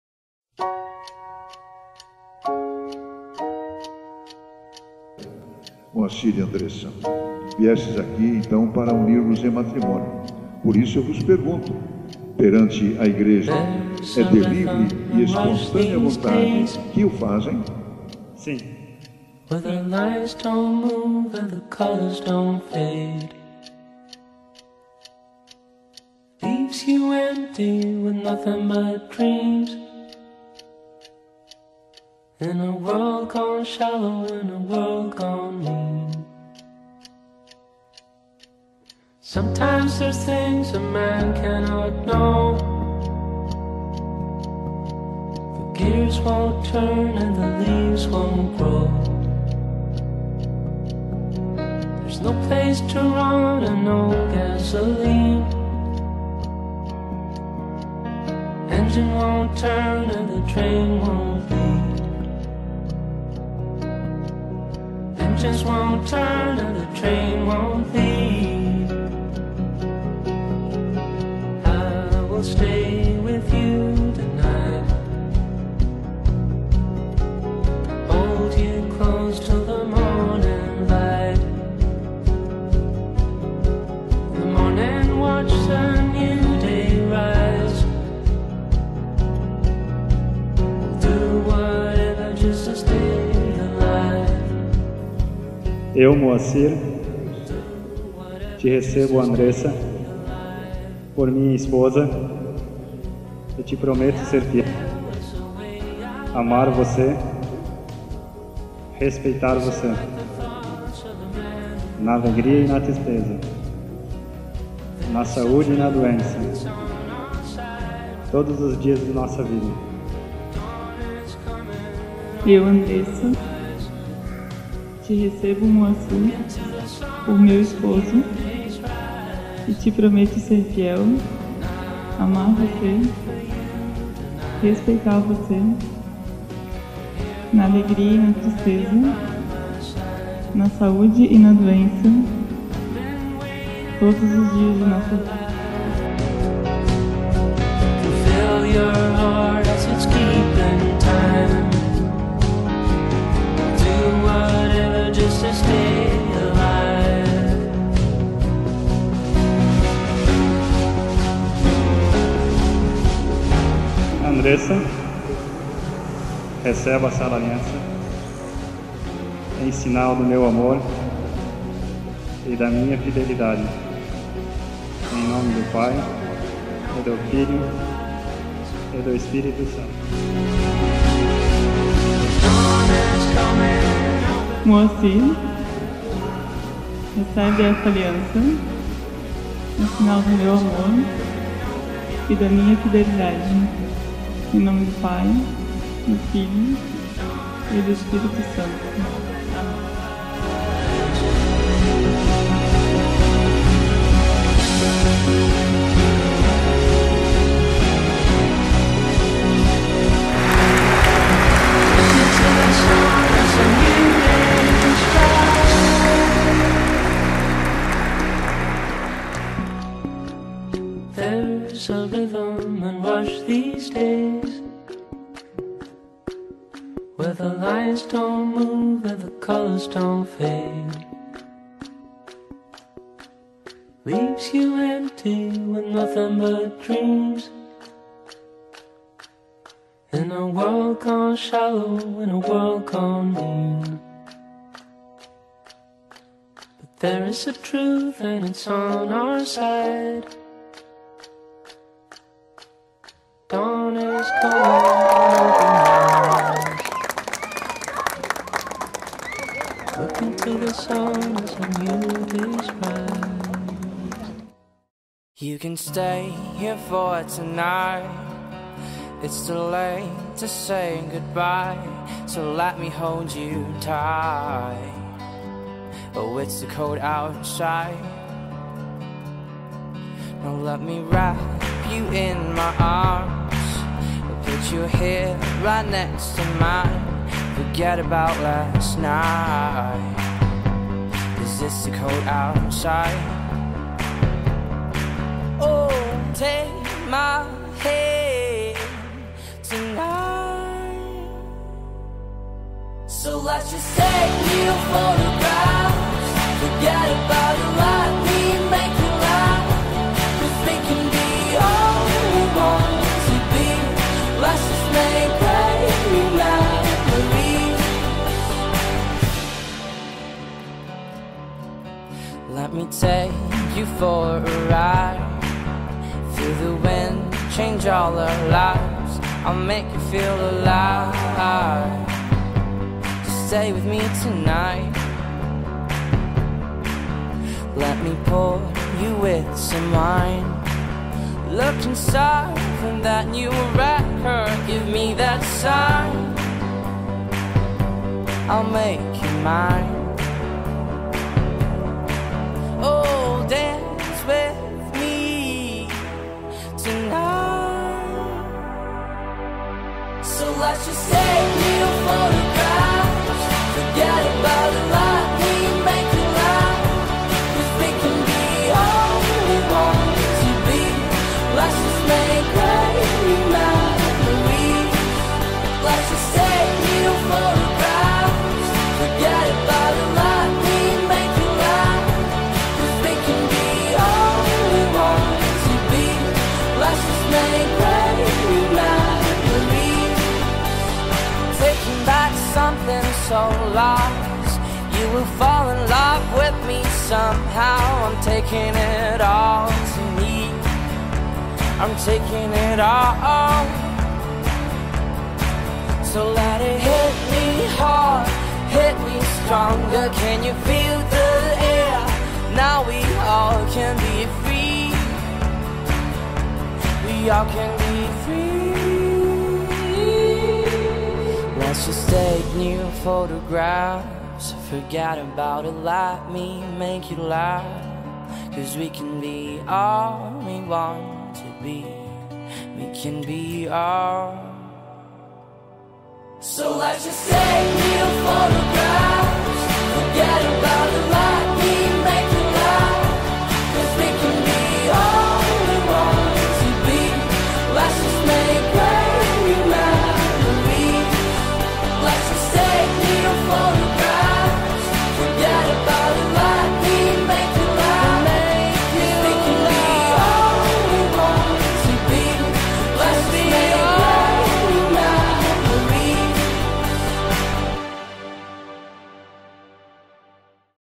Short Movie Andressa e Moacir
Clipe Short Movie do casamento do queridíssimo casal Andressa e Moacir, que se uniram em 15/10/2016. A cerimônia religiosa foi na igreja N. Sra. da Glória e a recepção no CSU Garcia. Parabéns ao casal!